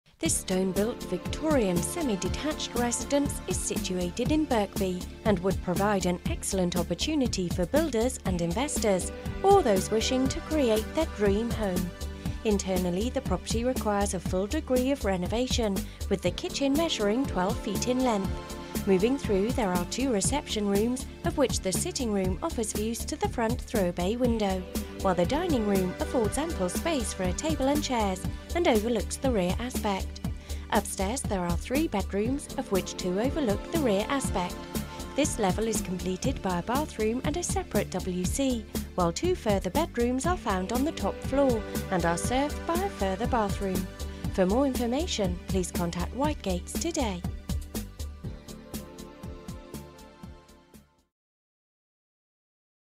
Birkby Hall Road, Birkby
* Offered for Sale by Tender with a dead line for formal Offers Over �175,000 by Friday 26th September, 2014
* A spacious stone built Victorian 5 bedroom semi detached property includes extensive rear garden plot
* Tired and run down requiring a full programme of renovation and modernisation